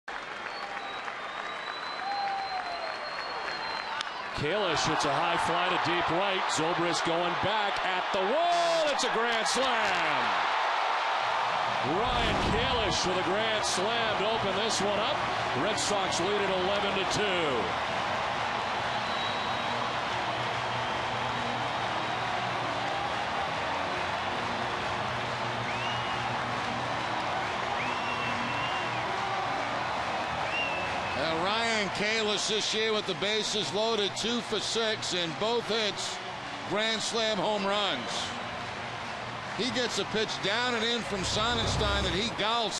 Ryan Kalish Hits Grand Slam Against Rays
Against the Rays in the bottom of the 4th inning on September 6, 2010, Ryan Kalish crushes a grand slam home run off Andy Sonnanstine.